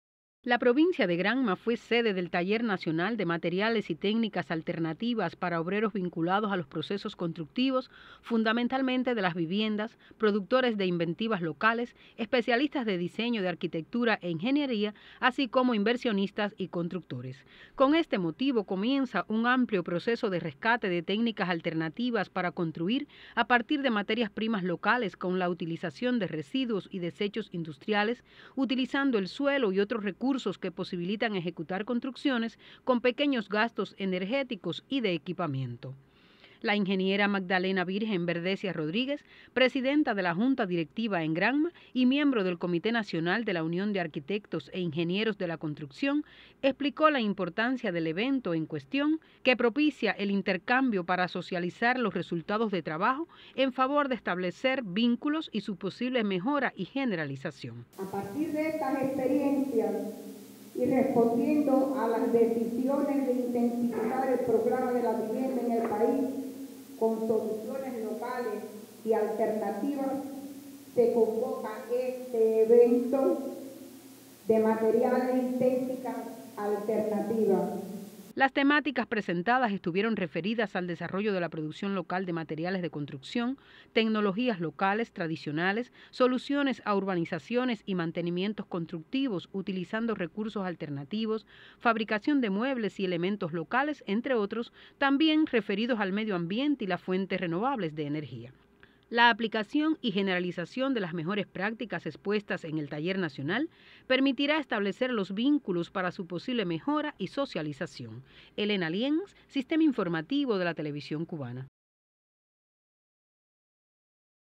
Buscan nuevas alternativas para la producción de materiales de construcción en Cuba
La Unión de arquitectos e ingenieros de la construcción desarrolló  en Bayamo un evento nacional para los profesionales vinculados a la producción  de materiales locales  mediante el empleo de técnicas alternativas.

El objetivo del taller  es generalizar las mejores prácticas en el uso de las materias primas locales, desechos industriales y recursos que posibiliten construir viviendas con pequeños gastos.